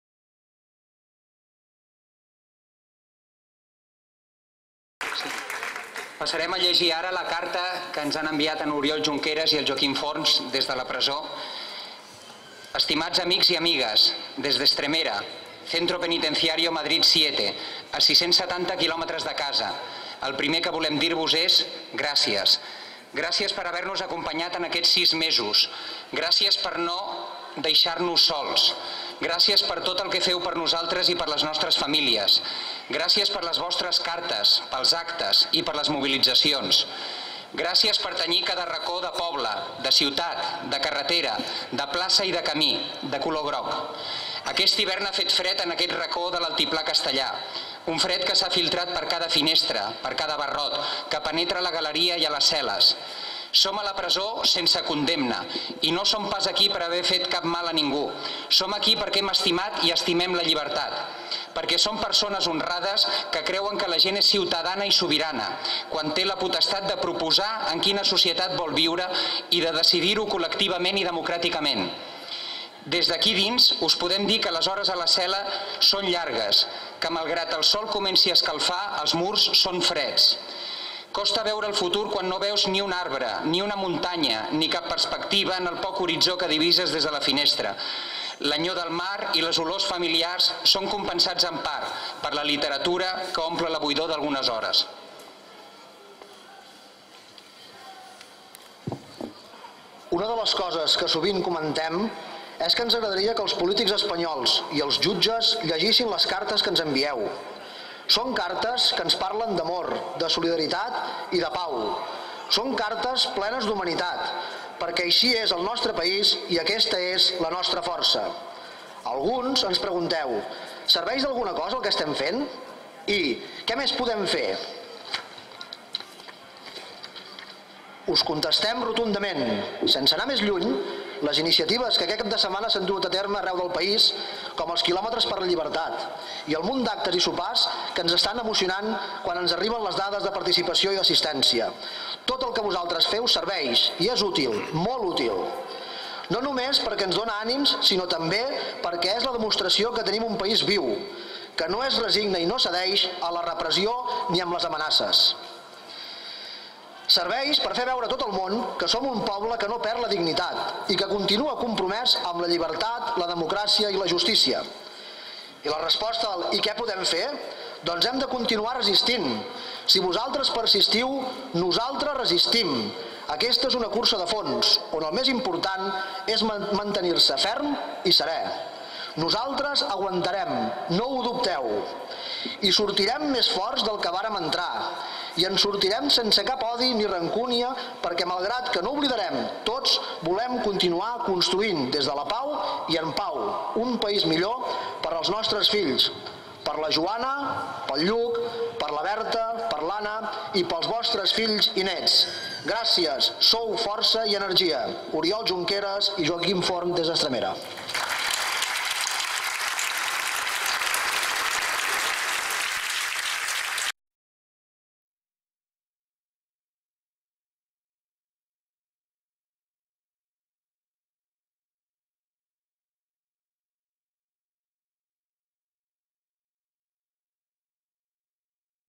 Lectura Carta Dels Jordis, Segrestats Polítics. 6 MESOS SENSE VOSALTRES. 02 05 2018.
SEGUIM ALCARRER:
 Avui fa 6 mesos de l’empresonament de l’Oriol Junqueras i el Joaquim Forn.
No defallim i seguim reclamant la seva LLIBERTAT!
19h a la plaça de la font!
Tarragona.